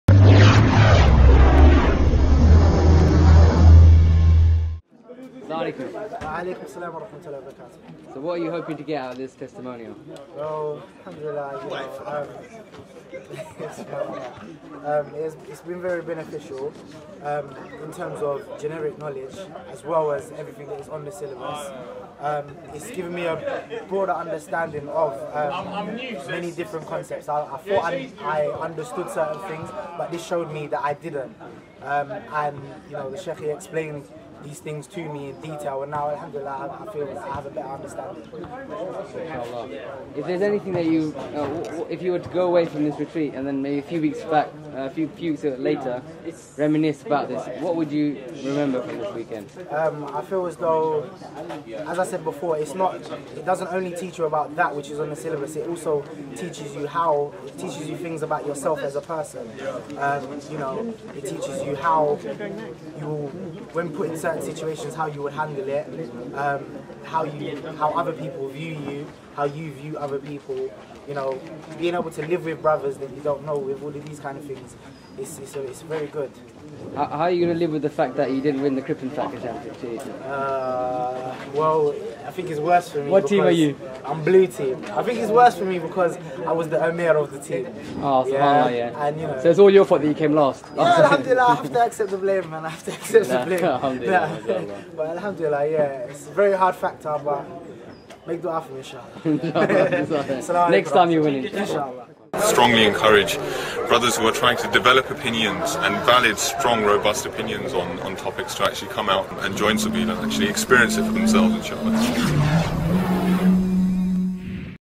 MRDF Sabeel Retreat Testimonial
Brother attending the Sabeel Retreat and shares his Awesome experience.